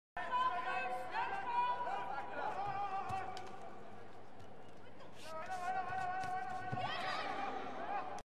Doha Masters 2021 🇶🇦 HAMADA, Shori Japan (JPN) 🇯🇵 STEENHUIS, Guusje Netherlands (NED) 🇳🇱
(Category -78 kg)